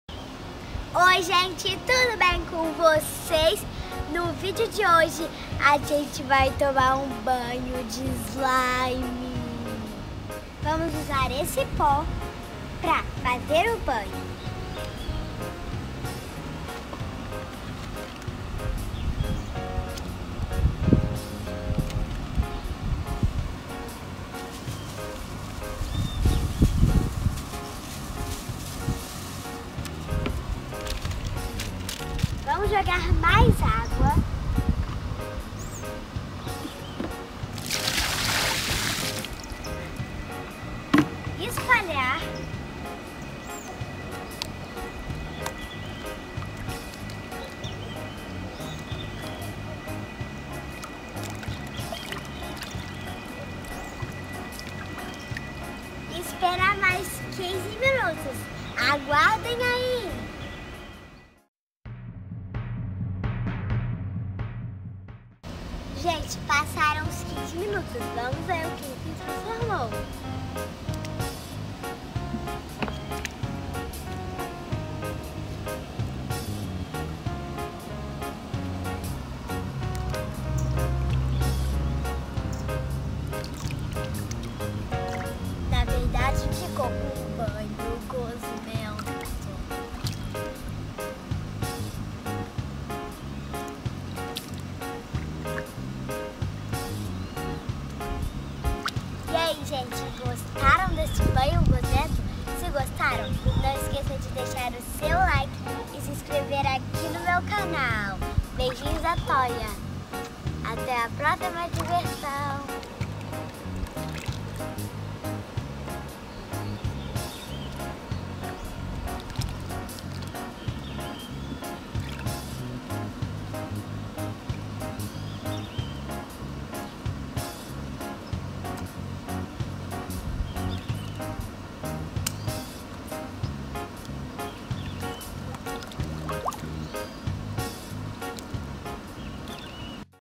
BANHO DE SLIME
NO VIDEO DE HOJE NOS VAMOS PREPARAR UM BANHO DE SLIME COM O SLIME BAFF, SERÁ QUE VAI DAR CERTO?